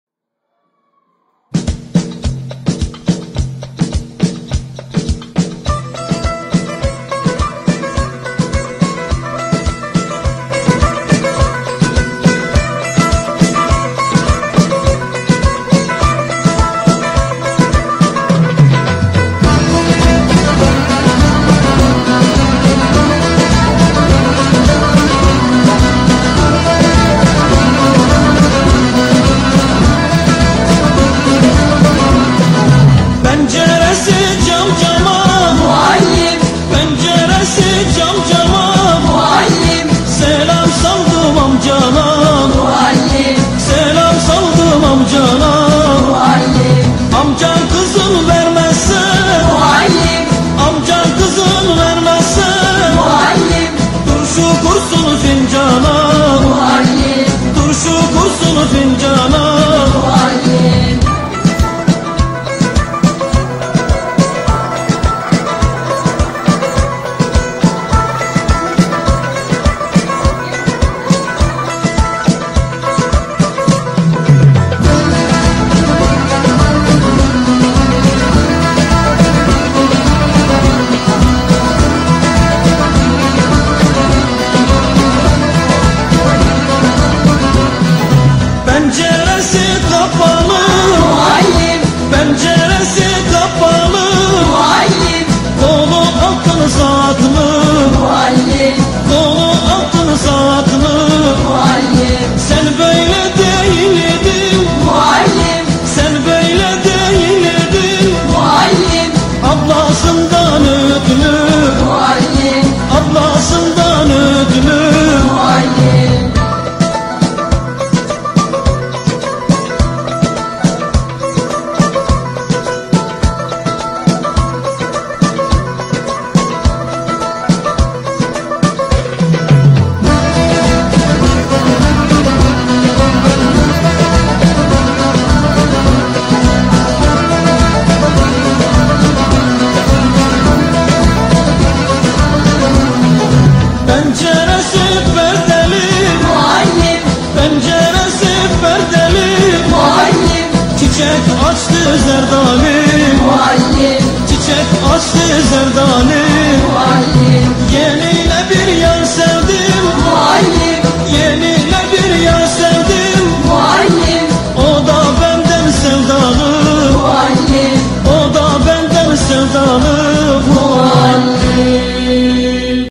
Muallim Of The Kunduzlu
Öğretmenler günü kutlamaları çerçevesinde, Bayat Kunduzlu öğretmen ve öğrencilerinden "muallim"